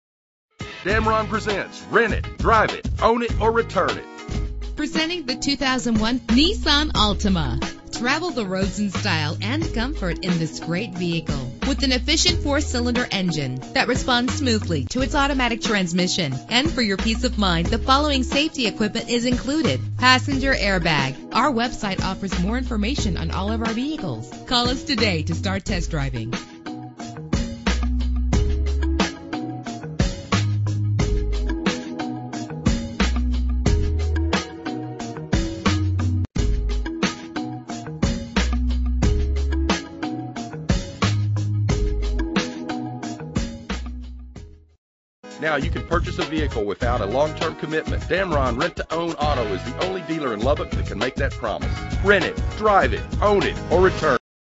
2001 NISSAN ALTIMA - Lubbock Texas
Year: 2001
Make: NISSAN
Model: ALTIMA
Trim: 4DR SDN
Engine: 2.4 liter DOHC SMPI 16-valve 4 cylinder engine
Transmission: 4-SPEED A/T
Color: GOLD
Mileage: 149606
Address:
2401 S.Loop 289